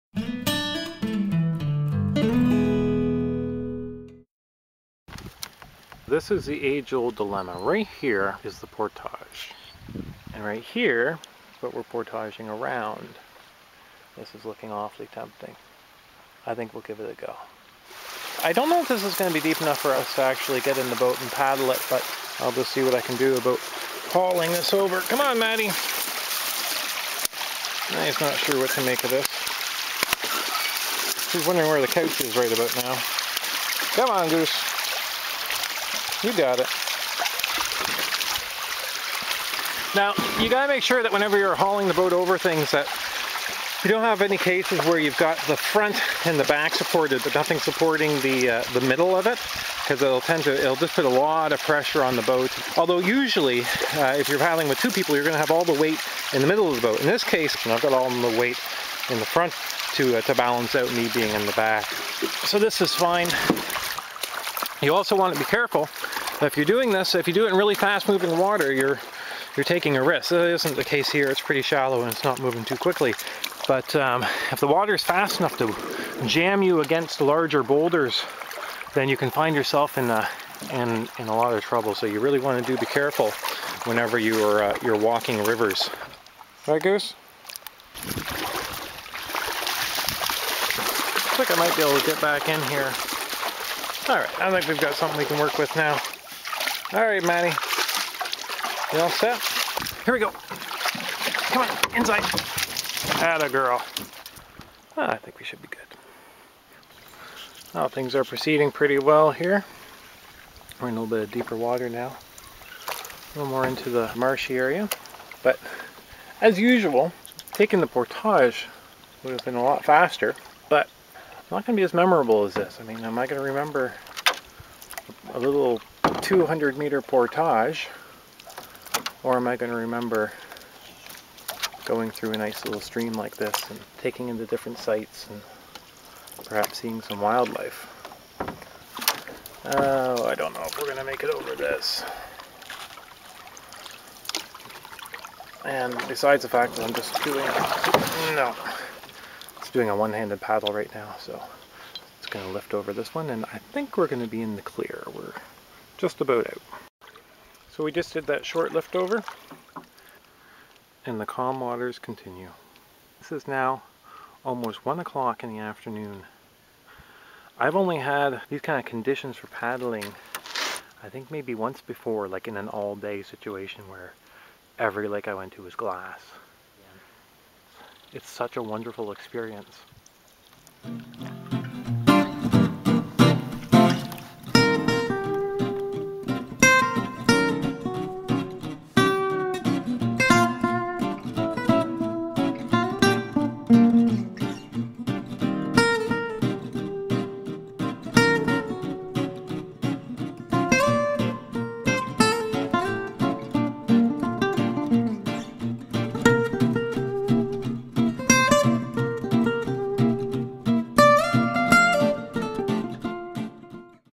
River Walkin' with the Goose (my dog)
Take a stroll with the Goose and me as we make our way down a shallow river to avoid a portage.